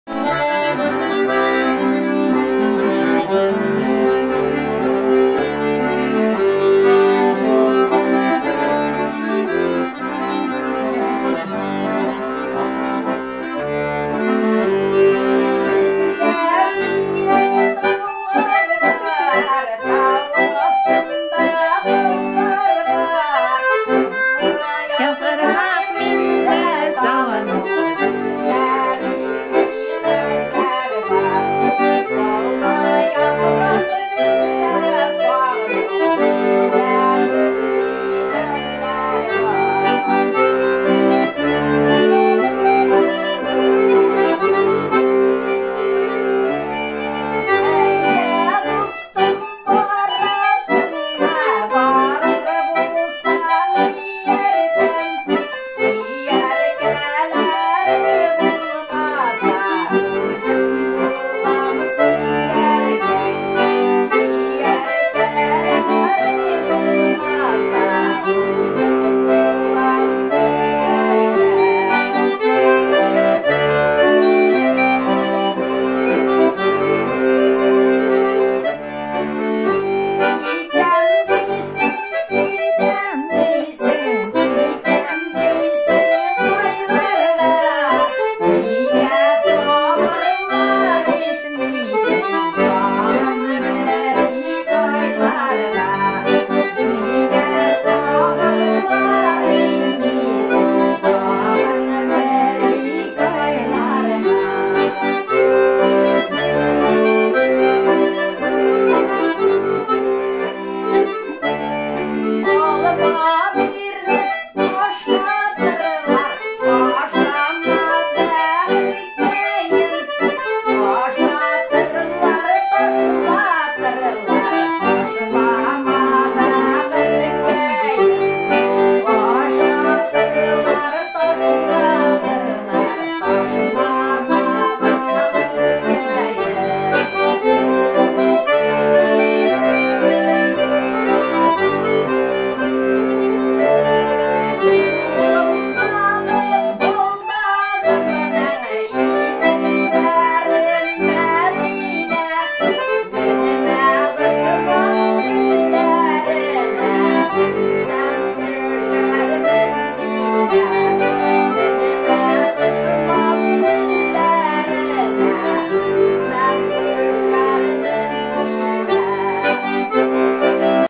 Халык жыры.
Модэрис абый Габдрахманов уйный.Жырлый Золхабирэ апа.90 яшь.Норма авылы.Балтач.